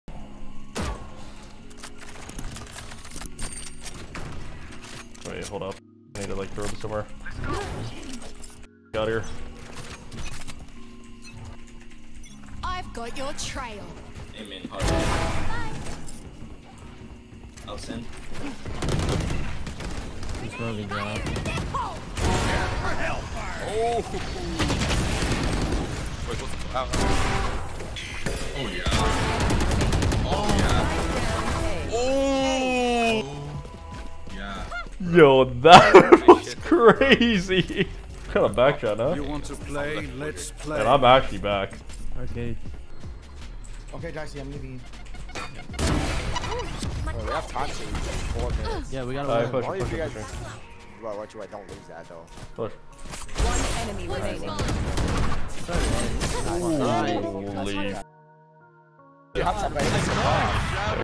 PROD throws his GUN at UNREACHABLE SPOT & hits the NASTIEST ACE Ever
PROD throws his gun to the roof because he knows he don't need it. Then he proceed to get the nastiest ace with his OP. This is actually crazy to watch. Prod also hits an insane turn around knife shot to win his premier. POG